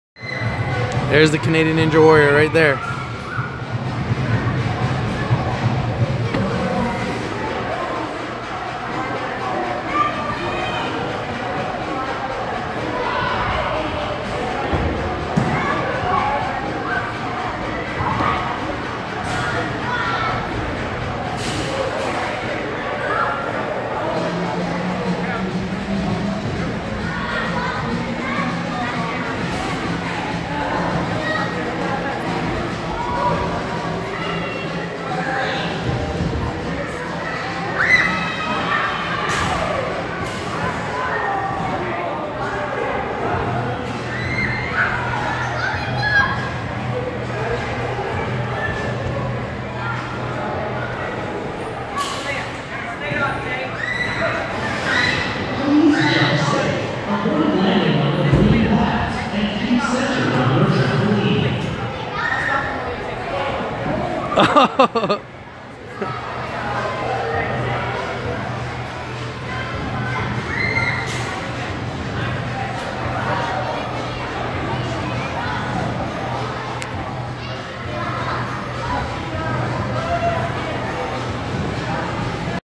Canadian Ninja Warrior is playing bubble soccer
Watch the Canadian Ninja Warrior play bubble soccer at extreme air